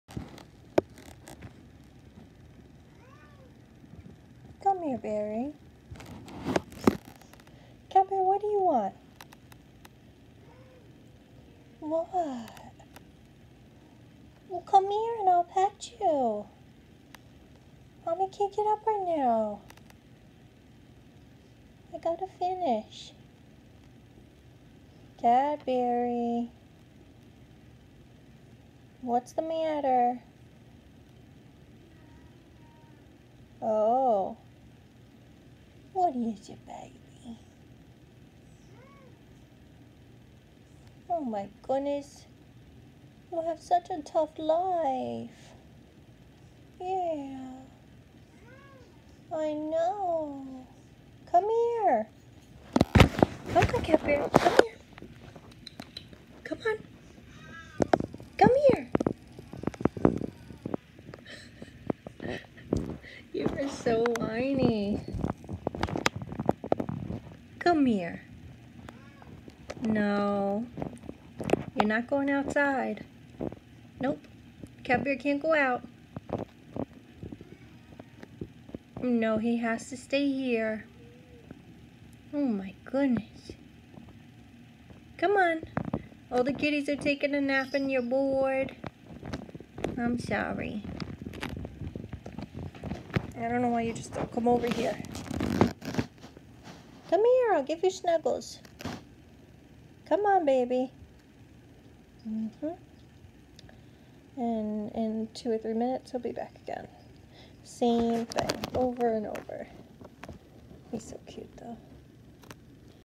Cadbury telling me all about it
Cadbury has a lot to say today! Cadbury is a seal point mink ragdoll, he is six years old.